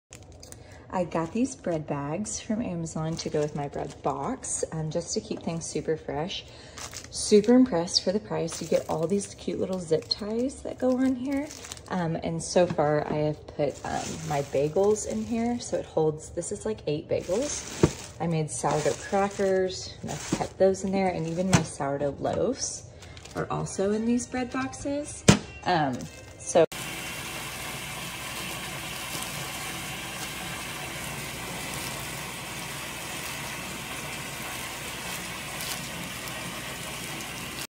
Review: Reusable Plastic Bread Bags for Homemade Bread - MMX-BB001
FUNSTITUTION Reusable Plastic Bread Bags for Homemade - 100 Pack Clear Bag with Ties For An Airtight Moisture-free Preservation and Storage- Loaf Home Bakers Bakery Owners

Review: Reusable Plastic Bread Bags for Homemade Bread - MMX-BB001IntroductionpStruggling to keep your homemade bread fresh for more than a day? If you're like me, you've probably faced the same issue countless times. That's why I want to share my experience with the Reusable Plastic Bread Bags for Homemade Bread, specifically the MMX-BB001 model. These bags have been a game-changer in my kitchen, and I believe they could revolutionize yours too./pThings to Consider before Buying Reusable Bread BagspBefore diving into the specifics of this product, let's consider why one would seek reusable bread bags. If you're someone who loves baking their own bread, you understand the frustration of seeing your hard work go stale within a day or two. These bags offer a solution to that problem, providing an airtight and moisture-free environment that extends the freshness of your bread./ppIdeal for avid bakers and eco-conscious consumers, these bags are not only convenient but also enviro